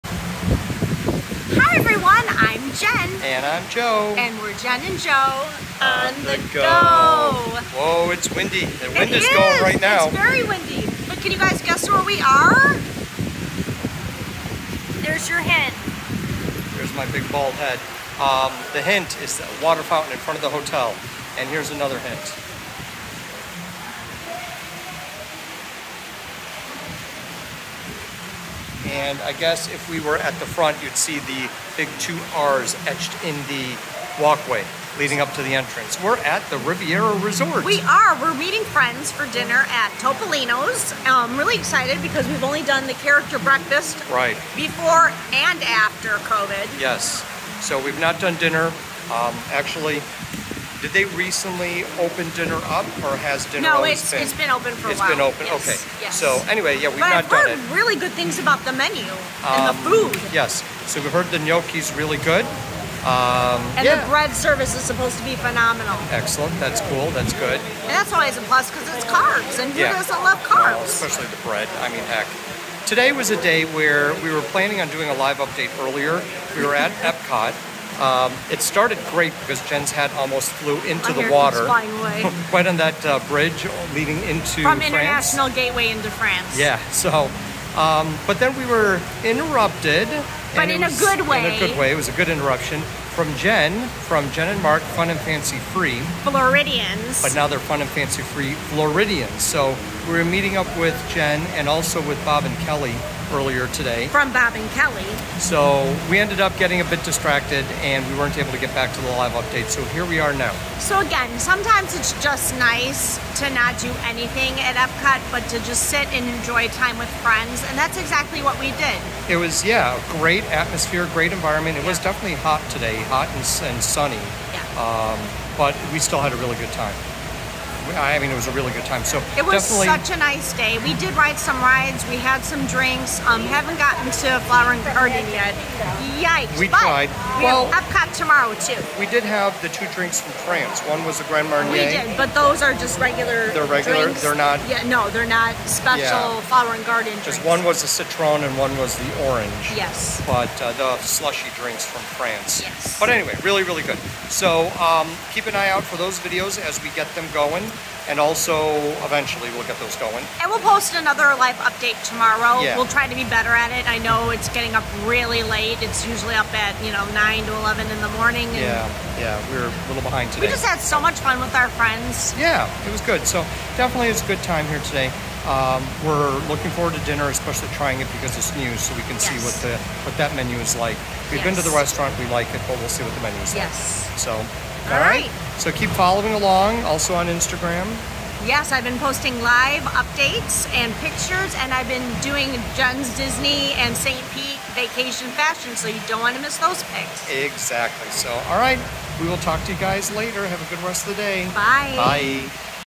Riviera Resort at Disney!
Live Update for Sunday, June 27, 2021.
We're at Disney's Riviera Resort, about to have dinner at Topolino's Terrace! So join us for a bite!